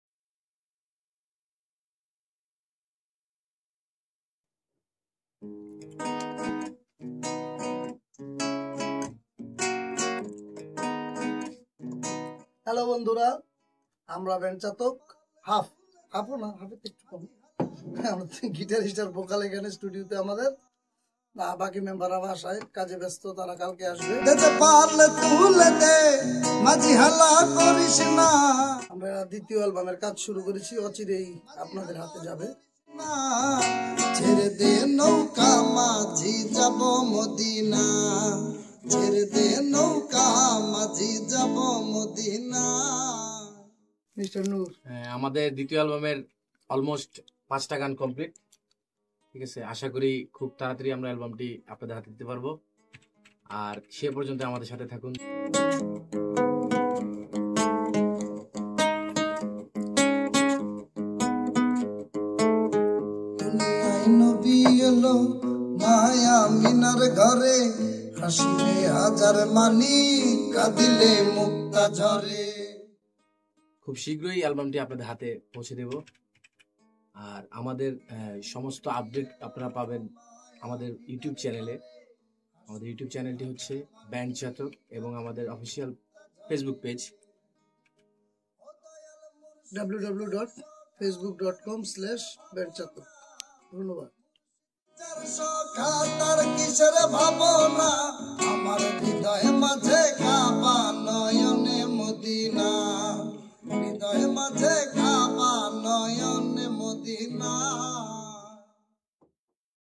Band Chatok 2nd Album Coming Soon | Promotional Video - 1 | 2017
Band Chatoks 2nd Album Arriving Soon .

Band Chatok Line Up :

Vocal : Shahriar Shams Kanady
Guitar : Polok & Nur Ali
Bass : Shanto
Drums : Limon

Promotional Video Made By Sabuj ahmed